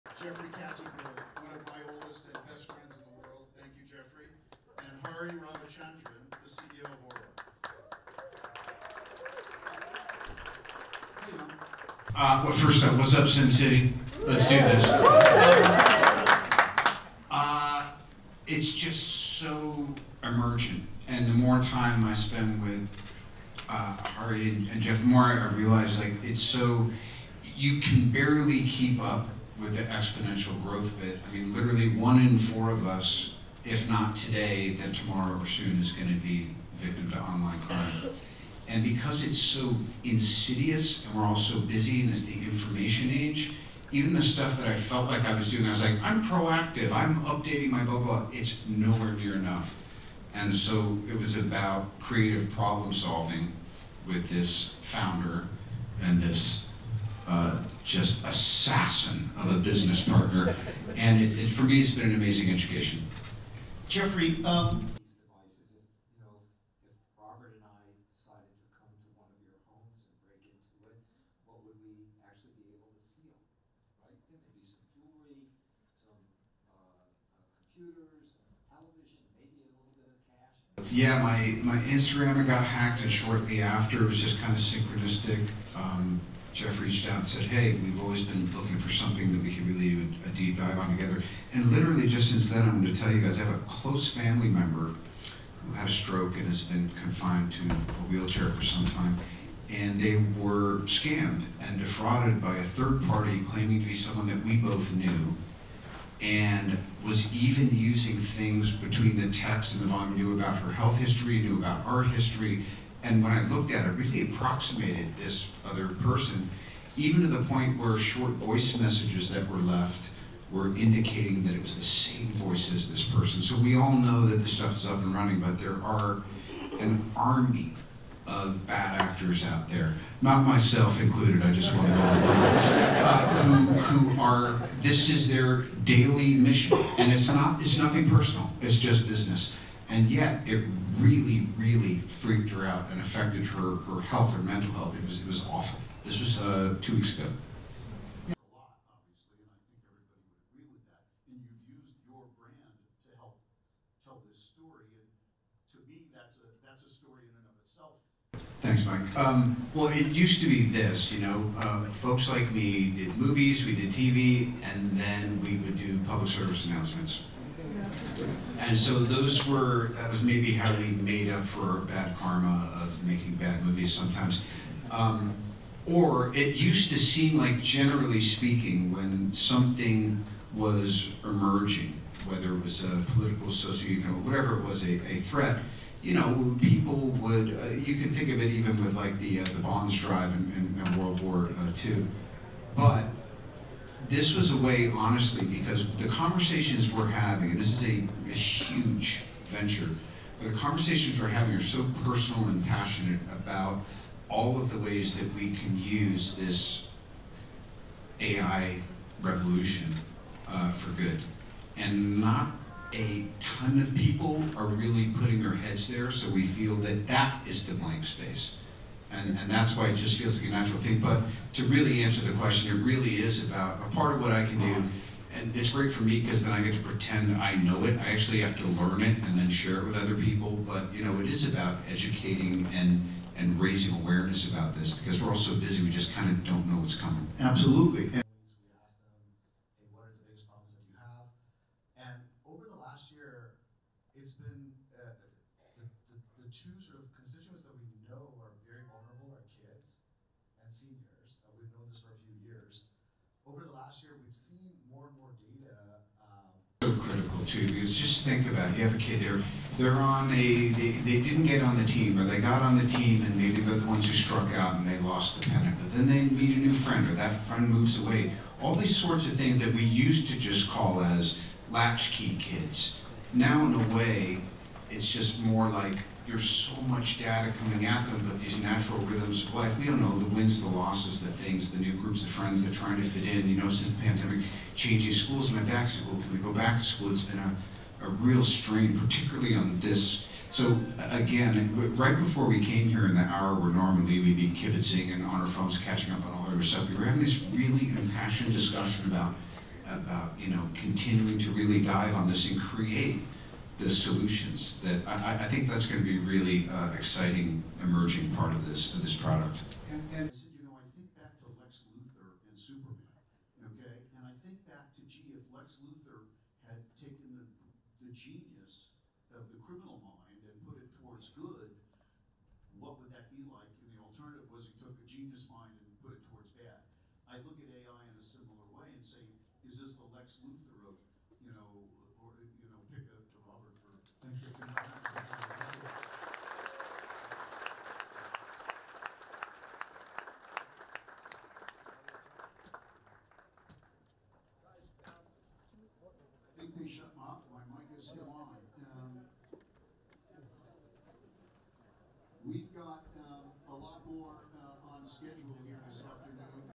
Robert Downey Jr at CES
RESTRICTION SUMMARY:
ASSOCIATED PRESS
Las Vegas, Nevada – 09 January 2024
1. Robert Downey Jr. walking on stage
2. SOUNDBITE (English) Robert Downey Jr., actor:
“Well first up hello Sin City. Let's do this. Uh, it's just so emergent. And the more time I spend with, uh, Harry and Jeff more, I realize, like, it's. So you can barely keep up with that exponential growth, but we literally one in four of us. If not today, then tomorrow soon is going to be online. And because it's so insidious, we're all so busy in the information age. Even the stuff that I felt like I was doing, I was like, I'm proactive, I'm updating my blah blah. It's nowhere near enough. And so it was about creative problem solving with this founder and this, uh, just assassin of a business partner. And it's for me, it's been an amazing education. “
3. Robert Downey Jr. listening
4. SOUNDBITE (English) Robert Downey Jr., actor:
“Yeah, my my Instagram. I got hacked in shortly after. It was just kind of synchronistic. Um, Jeffrey Stout said, hey, we've always been looking for something that we could really do a deep dive on together. And literally just since then, I'm going to tell you guys have a close family member who had a stroke and has been confined to a wheelchair for some time, and they were scammed and defrauded by a third party, claiming to be someone that we both knew and was even using things between the tax and the mom knew about her health history, knew about our history, and when I looked at it, really approximated this other person, even to the point where short voice messages that were left were indicating that it was the same voice as this person. So we all know that this stuff is up and running, but there are an army of bad actors out there, not myself included. I just want to know who you are. This is their daily mission, and it's not. It's nothing personal, it's just business. And yet it really, really freaked out and affected her for health or mental health. And that was two weeks ago“
5. Robert Downey Jr. listening to speaker
6. SOUNDBITE (English) Robert Downey Jr., actor:
“Um, well, it used to be this, you know, uh, folks like the the movies, we the TV. And then we would do public service announcements. And so those were was maybe how we made up for our bad karma of making bad movies sometimes. Um, or it used to seem like, generally speaking, when something was emerging, whether it was, uh, political says, you know, whatever was a threat, you know, when people would, uh, you can think of it even with like the, the bonds drive and World War Two. But this was a way, honestly, because the conversations were having this is a huge venture and the conversations were having so personal and passionate about all of the ways that we can use this AI revolution for good. And not a ton of people are really putting their heads there. So we feel that that is the blank space. And that's why it just feels like a natural thing. But to really answer the question, it really is about a part of what I can do. And it's great for me because then I get to pretend that I know it. I actually have to learn it and then share it with other people. But, you know, it is about educating and and raising awareness about this because we're all so busy, we just kind of don't know what's coming.“
7. Robert Downey Jr. on panel
8. SOUNDBITE (English) Robert Downey Jr., actor:
9. Robert Downey Jr. laughing
10. Leaving stage

STORYLINE:
Robert Downey Jr spoke about cyber security at the 2024 CES in Las Vegas Nevada. He was recently awarded a Golden Globe for his role in Oppenheimer.